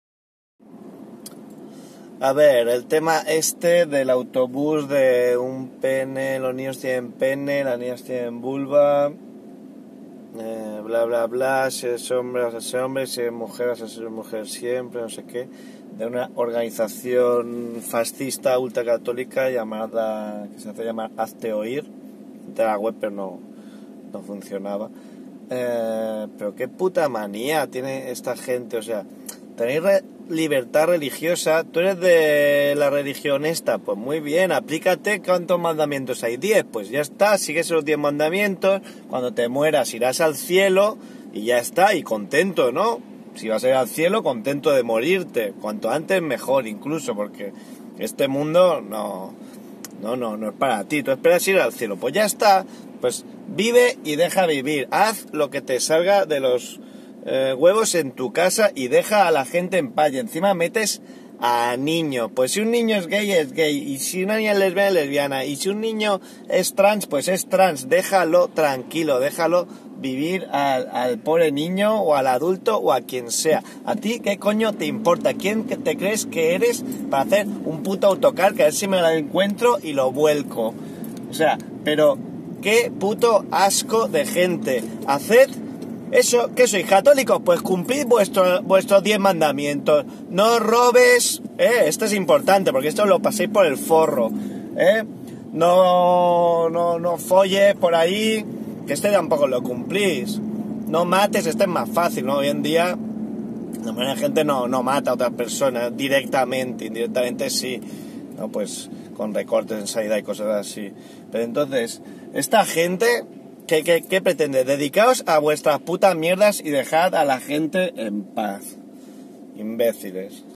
Mi opinión sobre el autocar de HazteOir, los niños tienen pene las niñas tienen vulva
La organización católica fascista HazteOir ha montado un autocar con la pretensión de recorrer España, con la publicidad "los niños tienen pene, las niñas tienen vulva. Si eres hombre eres hombre. Si eres mujer, seguirás siéndolo". Aquí mi opinión, siempre moderada.